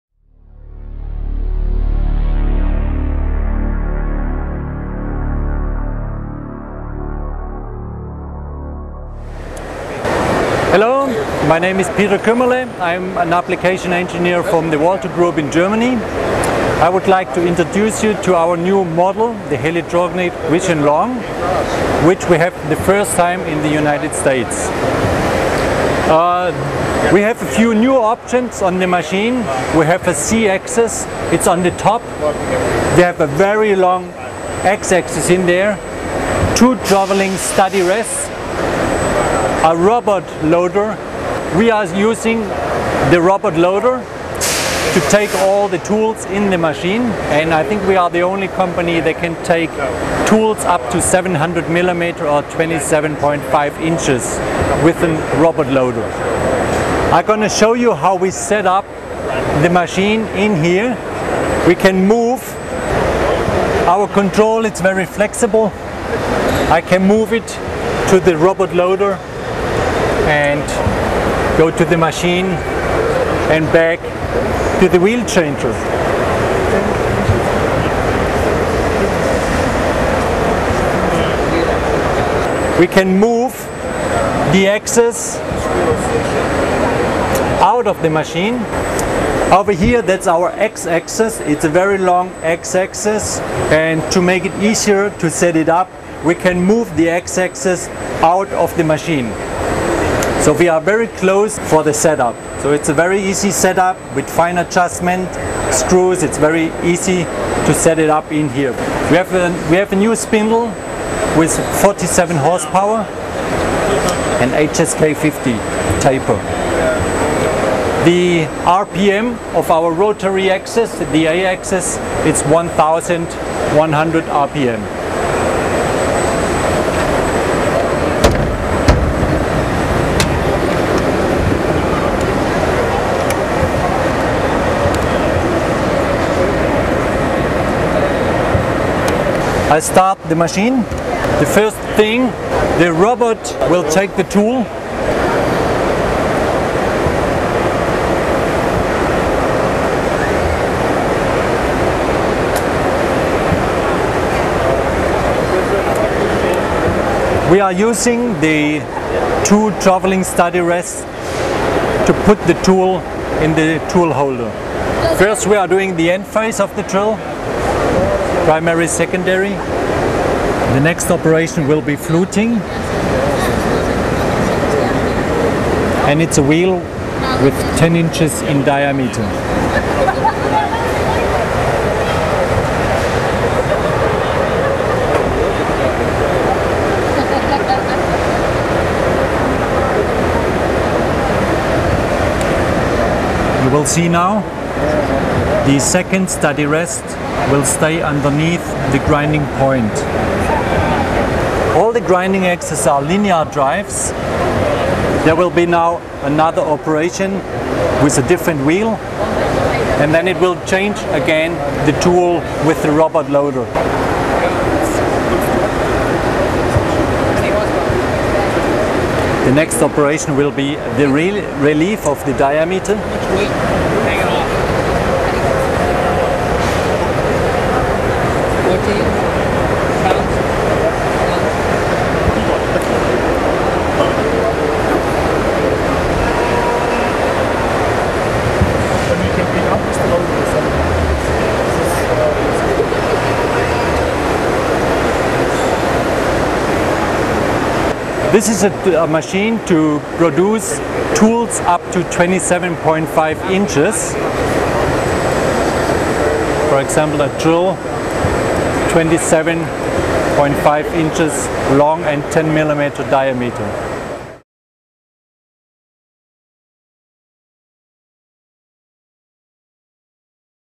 High-precision tool grinding for tools up to 700mm long
Peter Kummerle, applicationsengineer with United Grinding Technologies, offers an on-camera overview of the Helitronic Vision Long, which is said to expand production capabilities on tools up to 700mm long without exceeding the working envelope of the classical Helitronic Vision.